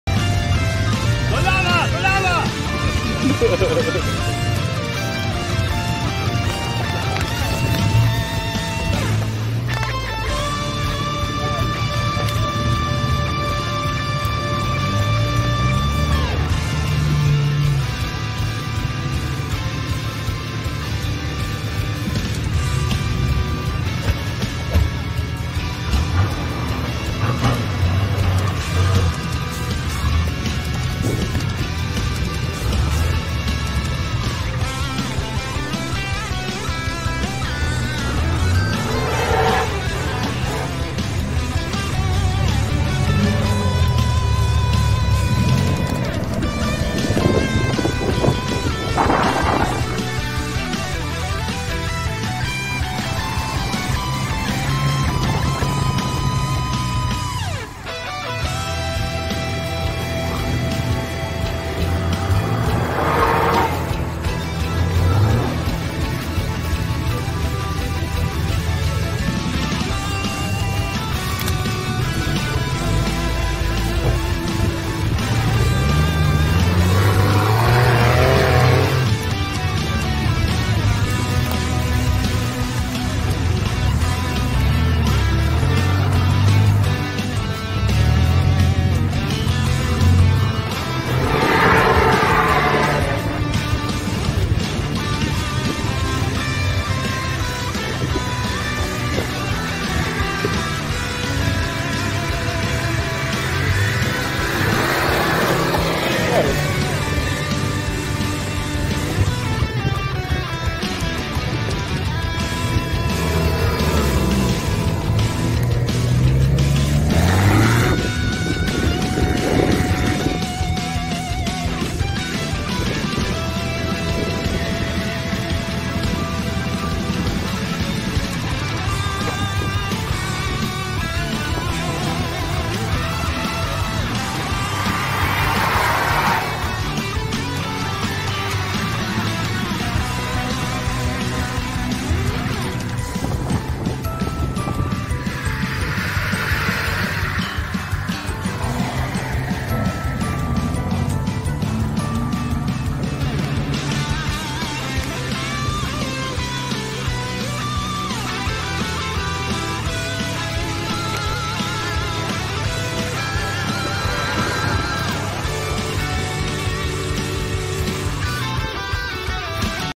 💪 Off-road vehicle - that's how you drive it!
An off-road vehicle that requires a special know-how when it comes to driving it. It is generally not complicated since anyone can do it. The particularity is just that this type of car being an all-terrain vehicle, its driving will be done according to the situation. Here are some tips on how to drive an off-road vehicle.
First, check that the tires are properly inflated. The correct air pressure of the tires should be indicated by the vehicle manufacturer on the doorpost, on the fuel tank door or in the glove box.
The key to successful off-road driving in any difficult situation is slowing down. Indeed, it allows a better control of the vehicle. Then you just need to be patient and calm enough and everything should be fine, whether it's night, in a storm, or in any extreme situation. Keeping your cool and not pushing the gas pedal is the key.

What is your favorite car?
What is your dream car?

Take care of yourself and your car!🔆